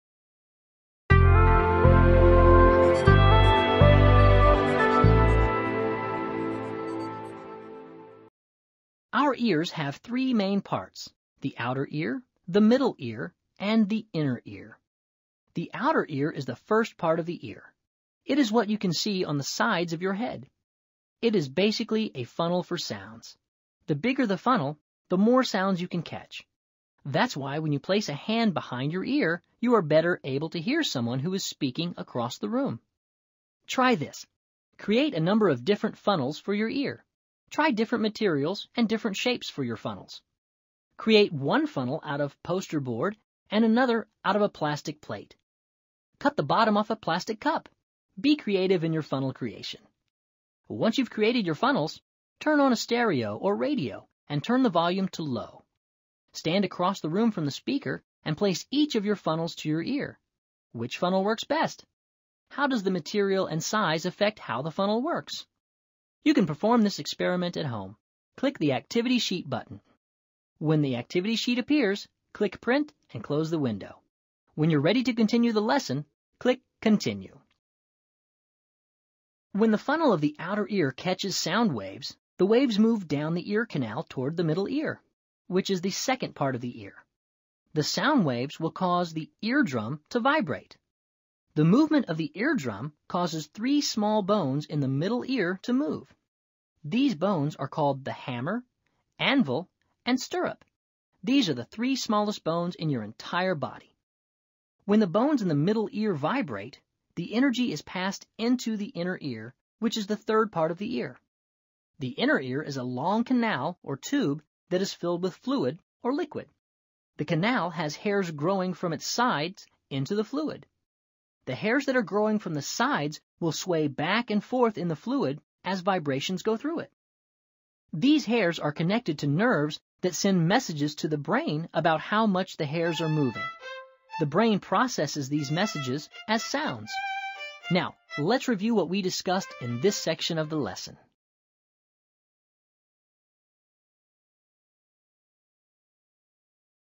Hearing
How we hear sound including information on the inner, middle and outer ear